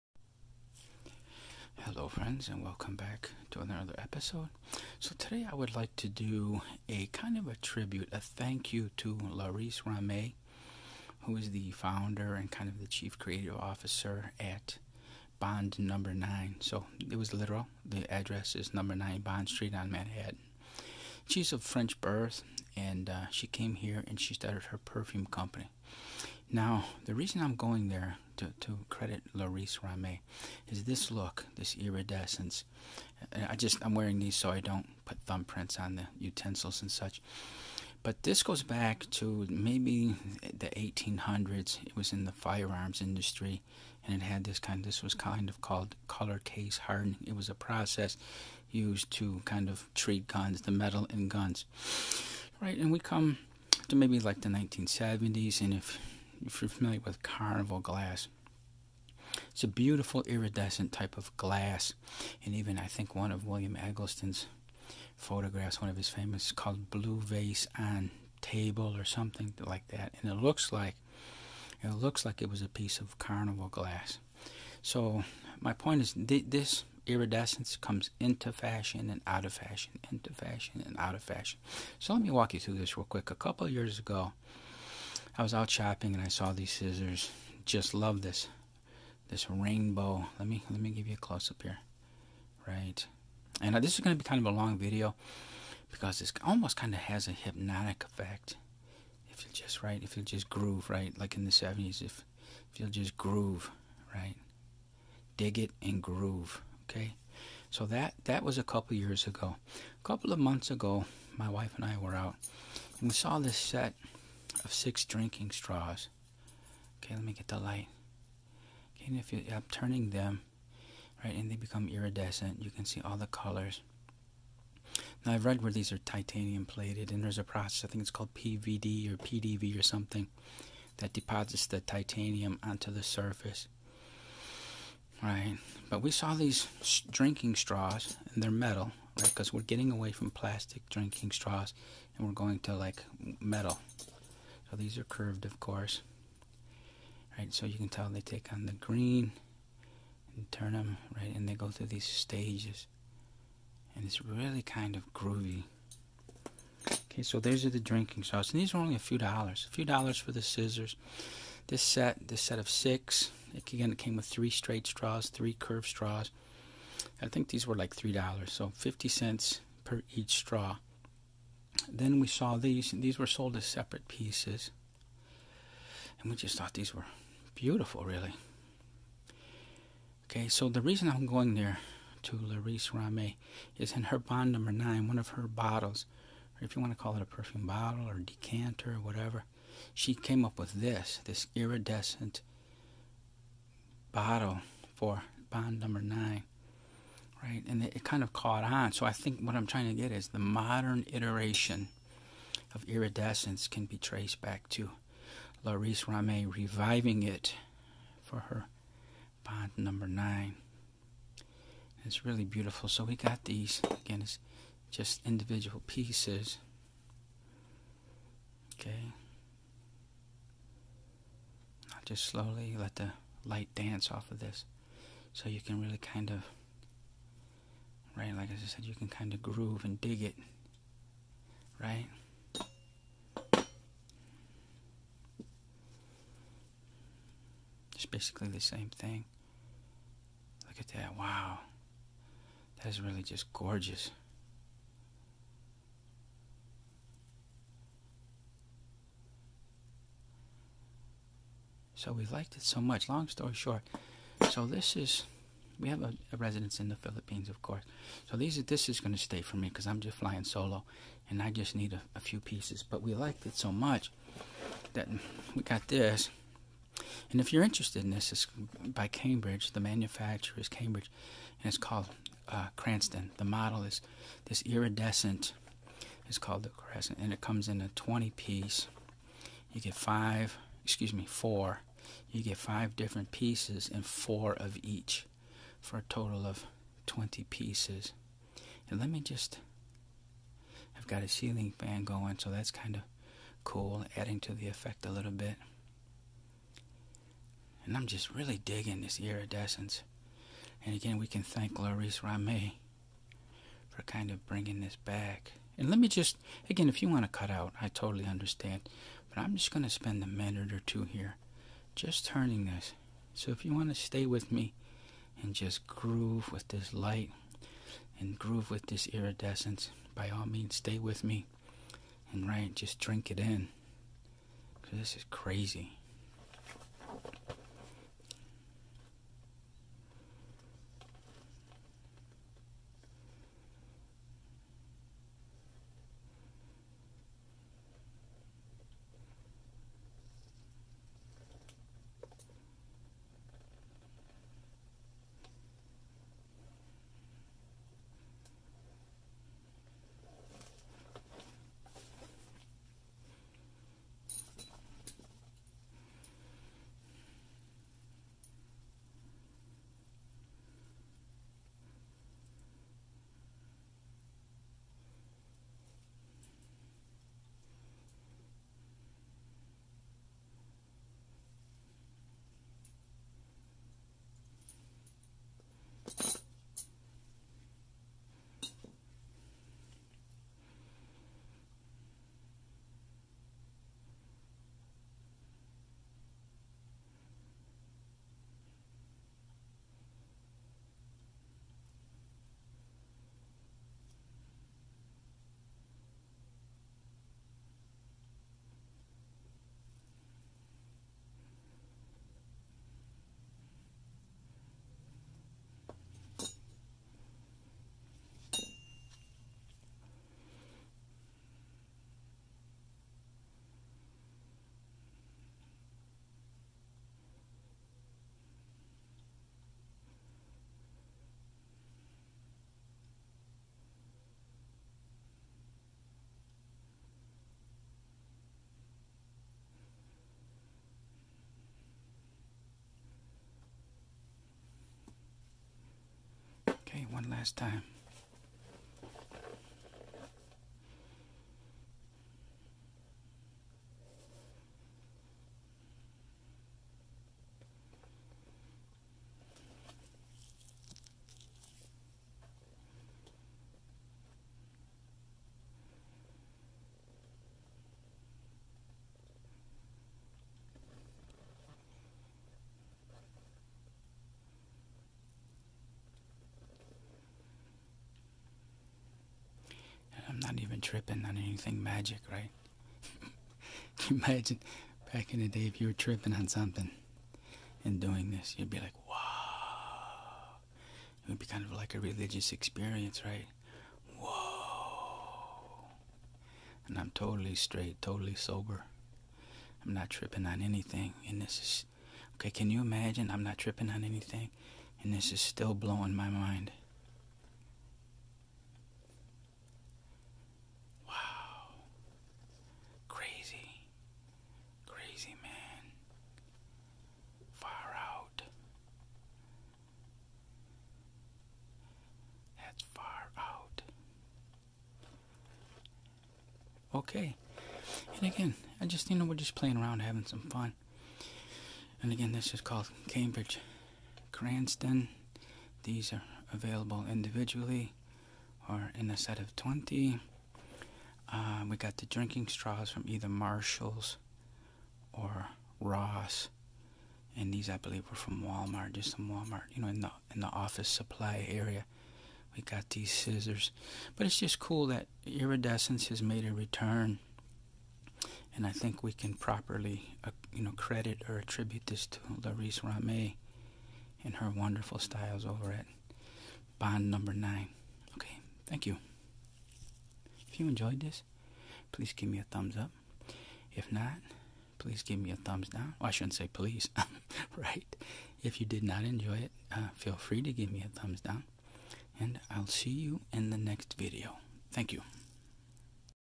Laurice Rahmé :: Bond No. 9 & The Return of Iridescence
Iridescence is making a comeback, attributable to Laurice Rahmé and her beautiful designs at Bond No. 9.